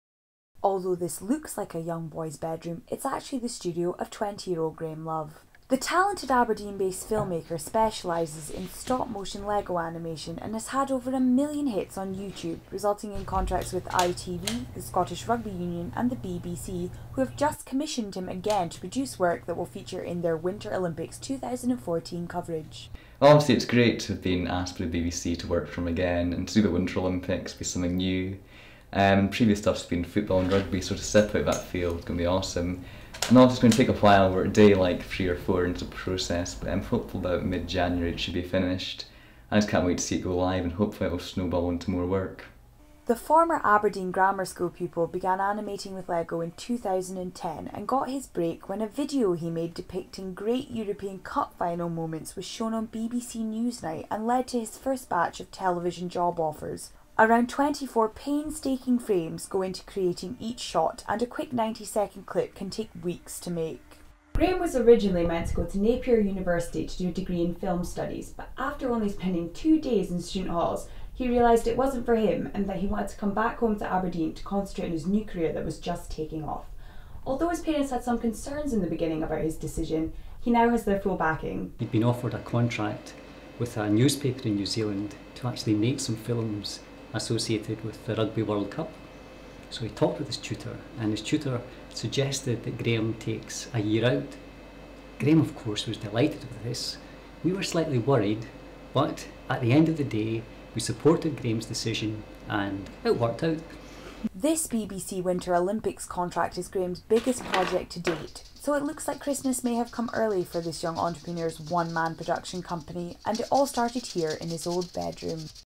Graham Love Lego Animation by Katy Spry
Interview and behind the scenes with the Aberdeen based animator. Graham Love as he works on his latest project for the BBC Winter Olympics 2014 coverage.